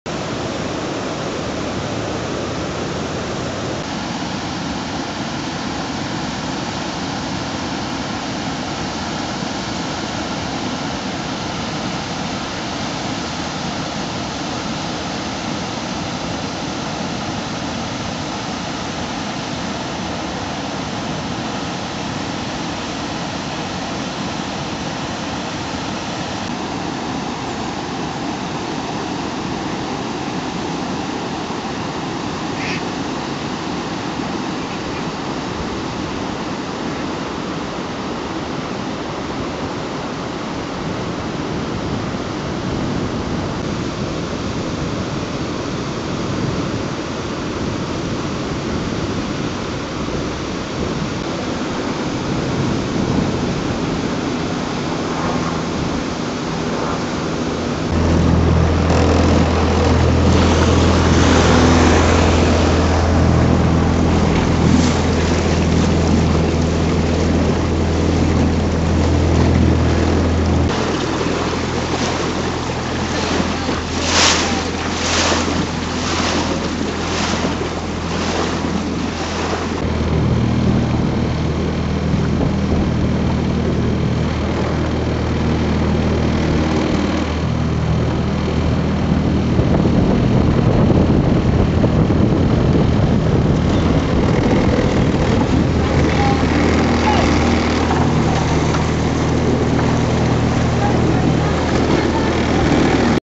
Norman 20
A short trip on the Thames from Hambledon lock to Henley reach